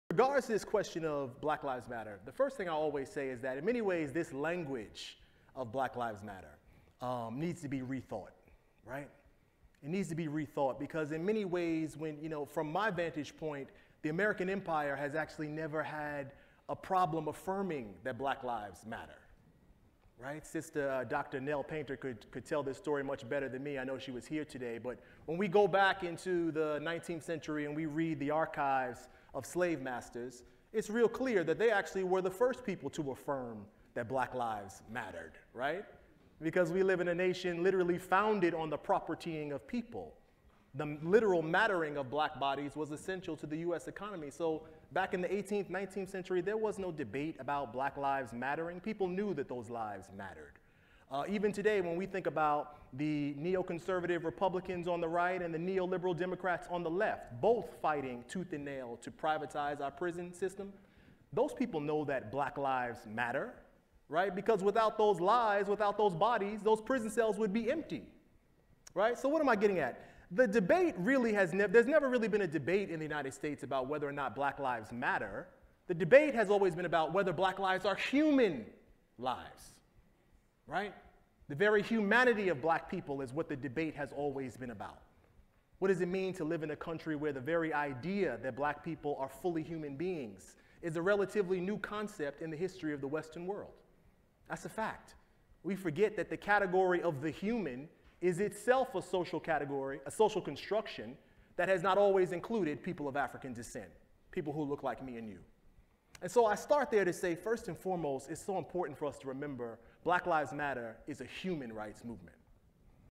Harvard Kennedy School Keynote Frank Leon Roberts
In this clip, taken from the keynote speech of the Harvard Kennedy School's 7th Annual Black Policy Conference, professor Frank Leon Roberts explains why black lives matter is a human rights movement.